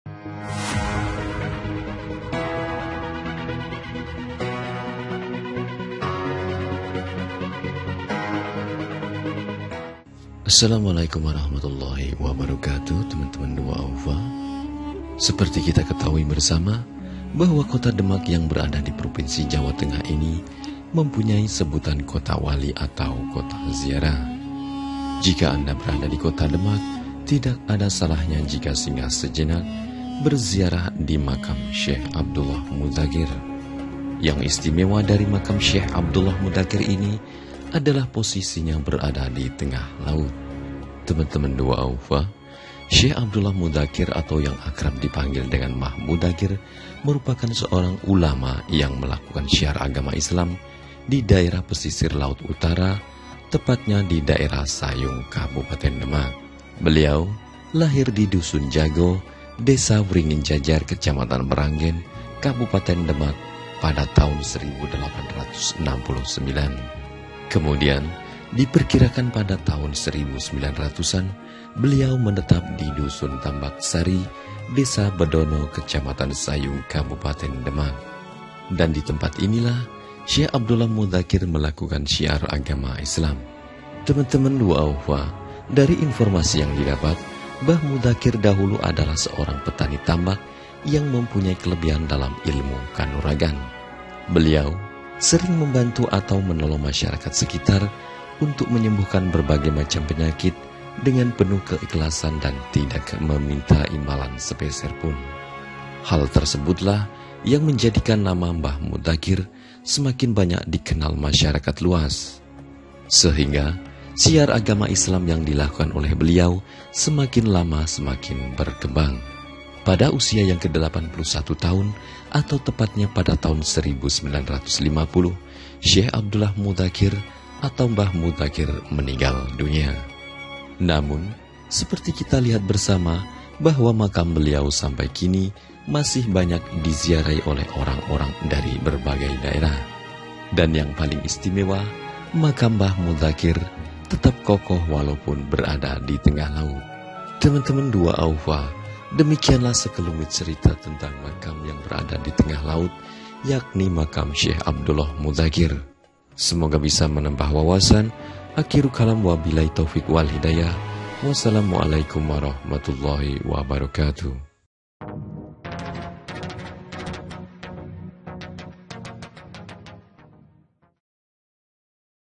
Aneh tapi nyata !! || Misteri MAKAM MENGAPUNG DI TENGAH LAUT
Syekh Abdullah Mudakkir atau yang akrab dipanggil mbah Mudzakir merupakan seorang ulama yang melakukan syiar agama Islam di daerah pesisir laut utara tepatnya di daerah Sayung kabupaten Demak.
Dan ditempat inilah syekh Abdullah mudzakkir melakukan syiar agama islam.